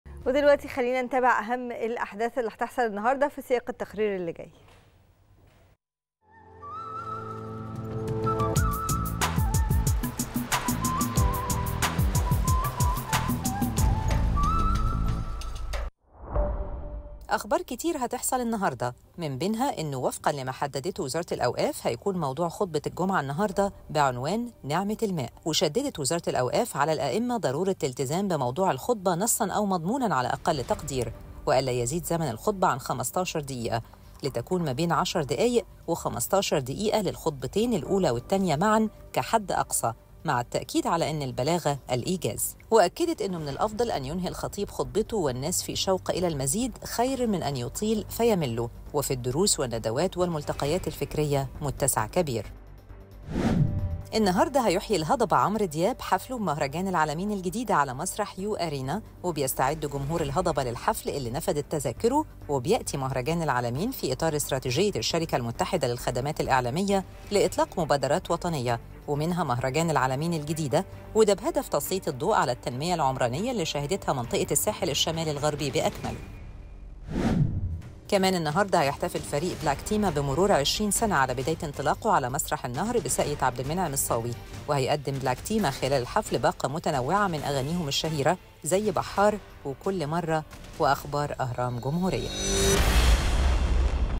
الهضبة عمرو دياب يحيي حفله بمهرجان العلمين الجديدة على مسرح u arena
يحدث اليوم.. الهضبة عمرو دياب يحيي حفله بمهرجان العلمين الجديدة على مسرح u arena